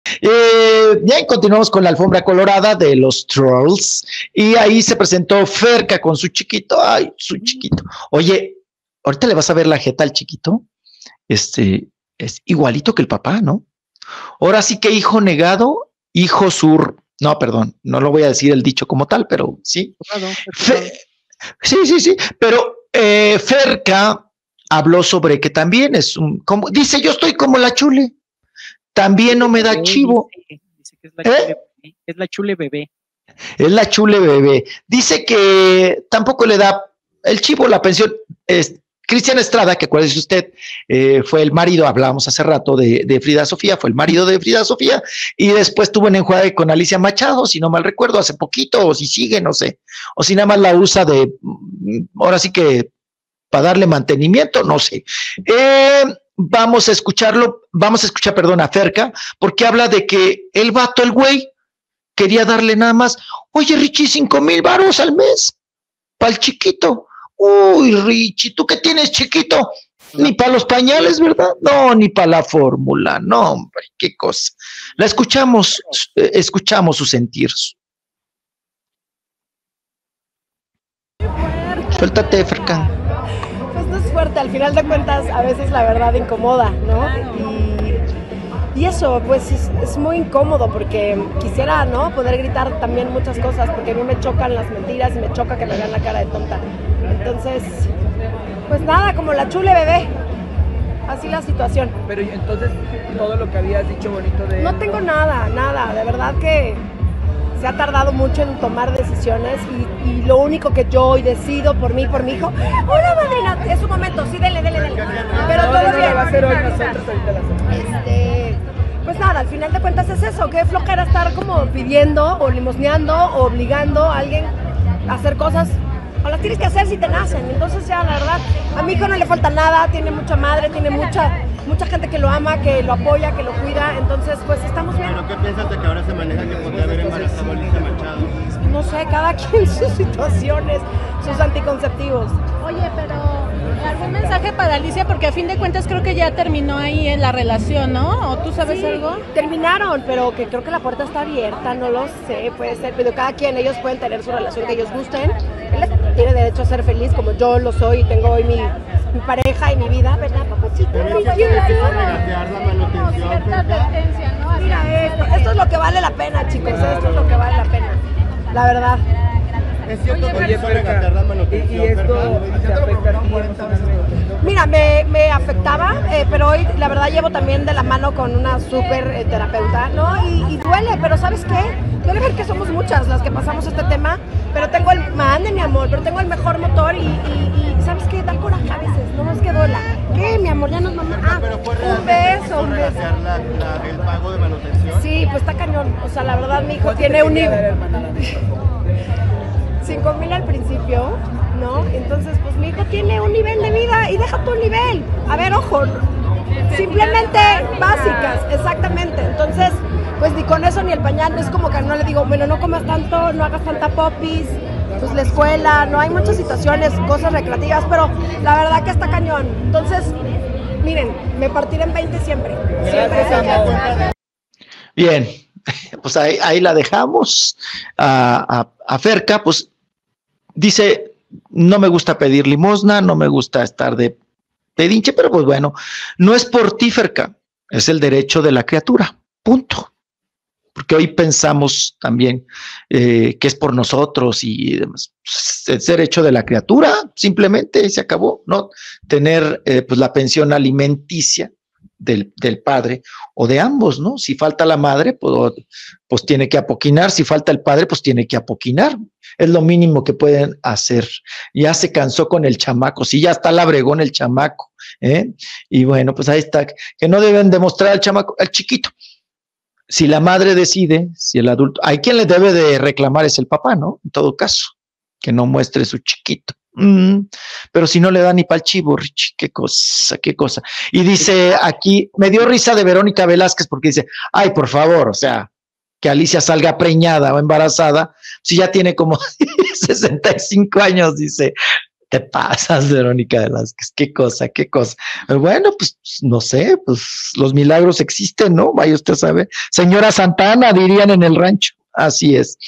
FERKA NO PIDE LIMOSNA POR EL CHIQUITO
Ferka dice que su chiquito tiene mucha madre y no necesita del dinero de su padre para sacarlo adelante... ¿Te lo perdiste?

Entérate de esta y otras noticias del espectáculo viendo nuestro programa de Lunes a Viernes 10:30 am (Hora CDMX).